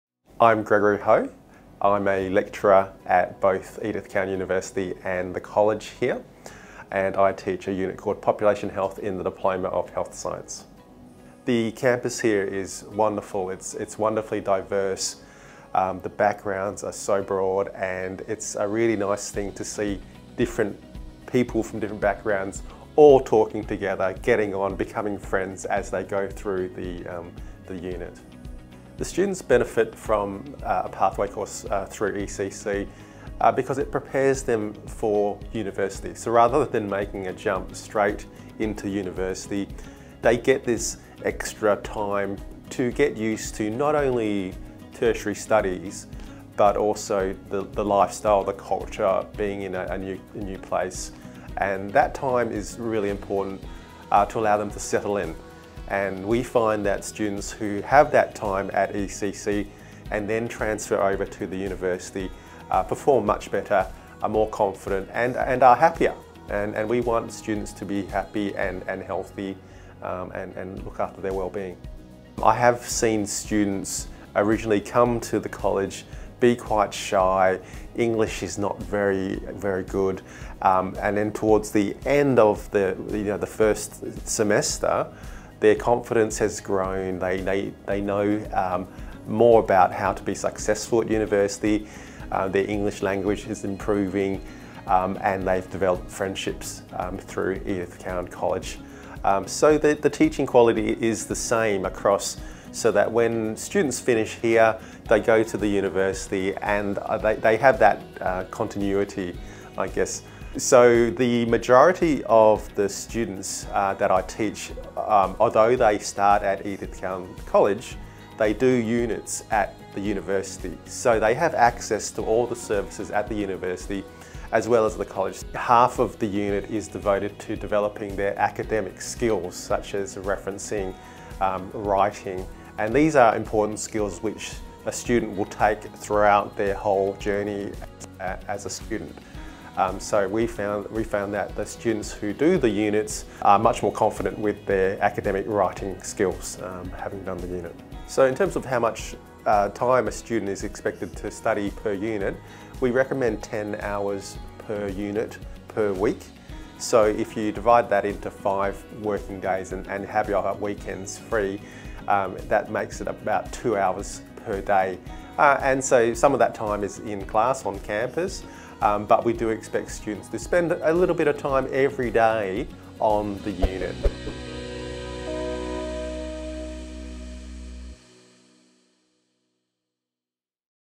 ECC Lecturer interview - Gregory
Gregory, Lecturer at Edith Cowan College, provides advice for our students.